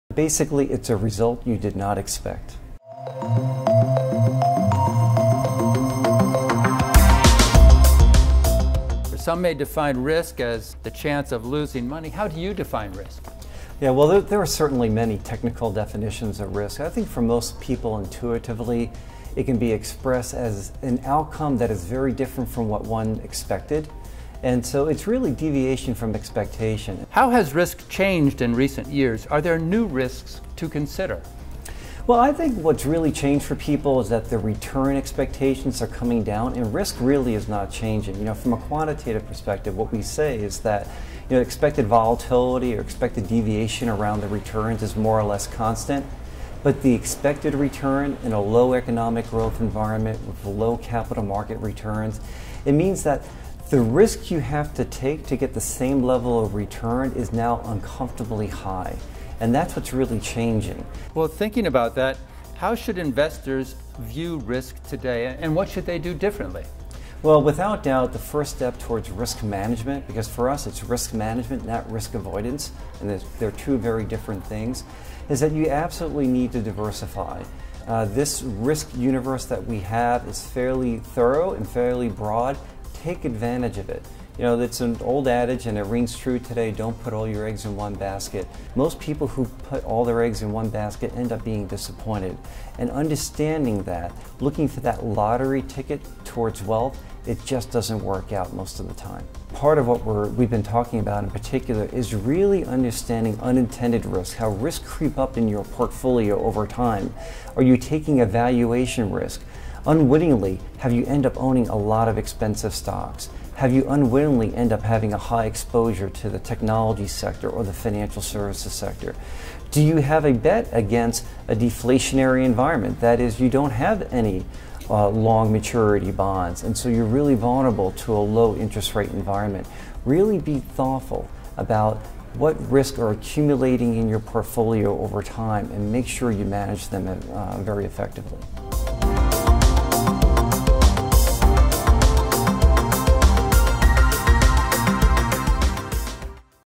Risk Perspective
Risk amounts to unexpected results and risk levels should be carefully monitored in portfolios, says Bob Browne of Northern Trust.